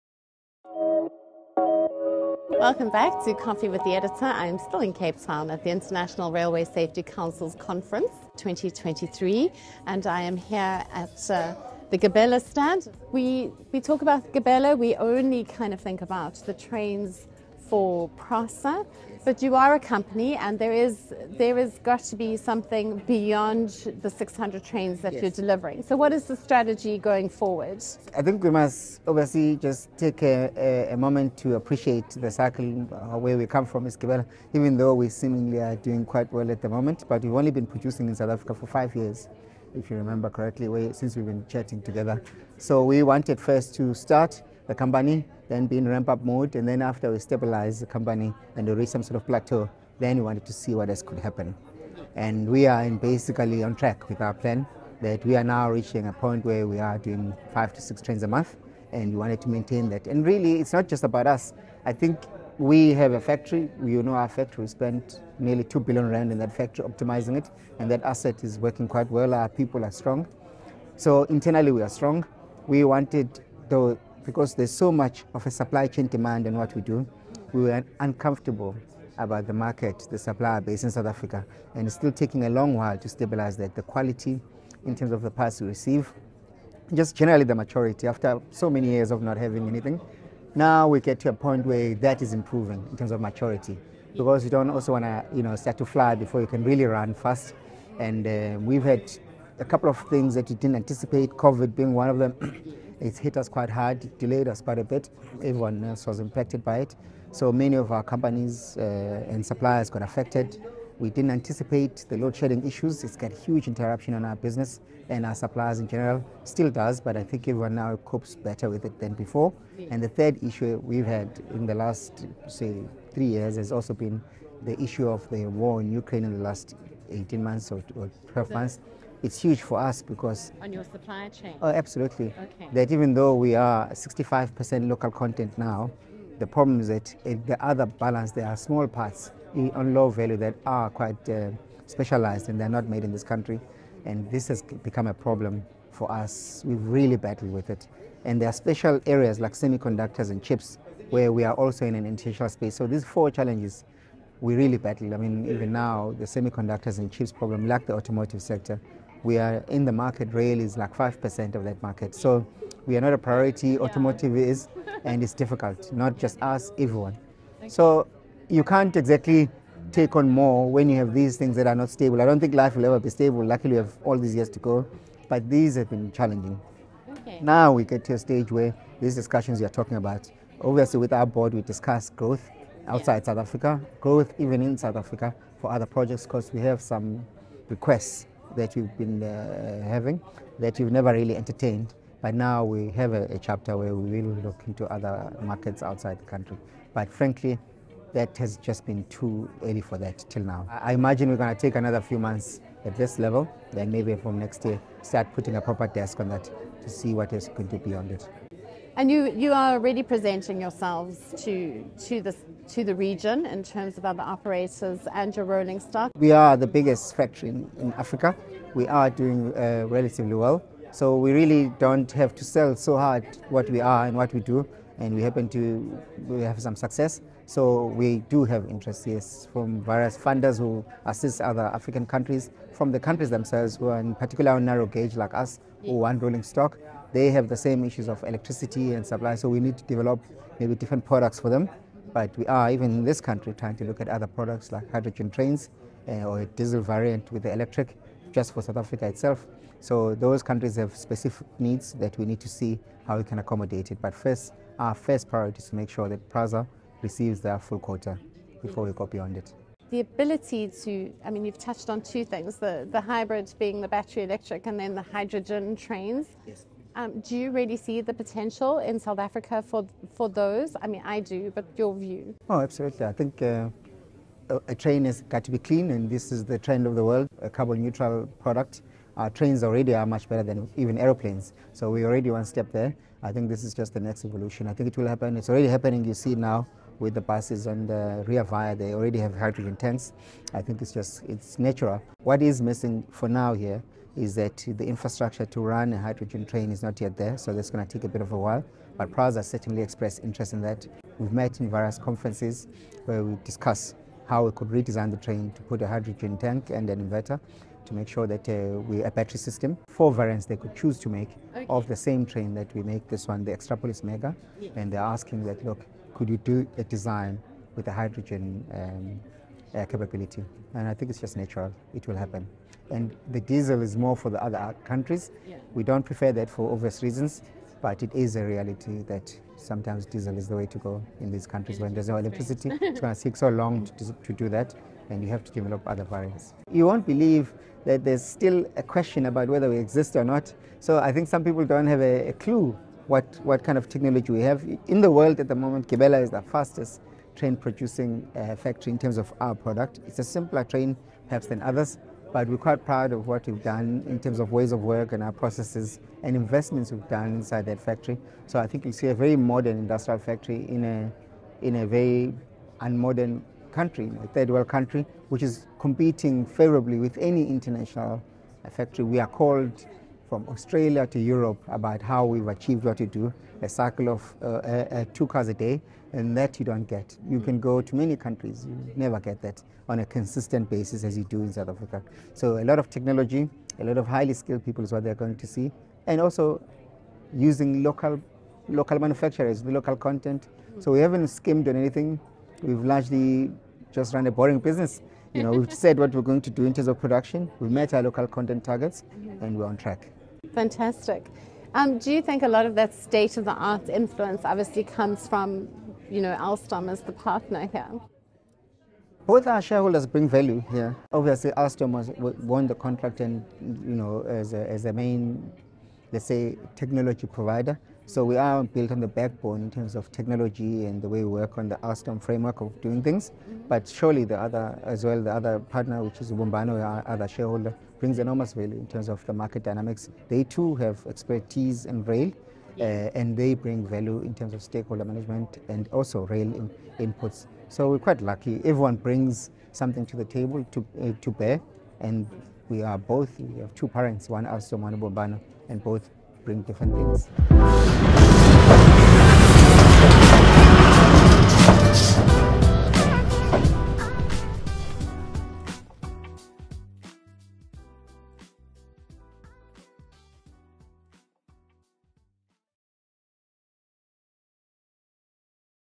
In-Depth with Hector Danisa: Gibela's Future Beyond PRASA Trains
In this engaging episode of "Coffee with the Editor," recorded at the International Railway Safety Council's Conference 2023 in Cape Town, we sit down with Hector Danisa, CEO of Gibela Rail Transport Consortium. The focus is on exploring what lies ahead for Gibela after delivering 600 trains for PRASA.

Delve into Hector's insights on the company's growth, the challenges it faces, and its strategic vision in the face of global disruptions like COVID-19, load shedding, and the Ukraine war. Discover the advanced Gibela factory in South Africa, a symbol of industrial prowess in a developing country, which has achieved peak production and set new standards in train manufacturing.

Learn about Gibela's innovative strategies for overcoming semiconductor shortages and their plans for expansion both within South Africa and internationally. Hector also sheds light on Gibela's impactful role in the African rail industry, their state-of-the-art factory operations, and ambitious projects like hydrogen and hybrid trains. He emphasises the importance of local manufacturing, technology partnerships, and effective stakeholder management in the complex landscape of the rail industry.

Don't miss this insightful discussion that highlights the future of rail transport in Africa and the critical role Gibela plays in shaping it.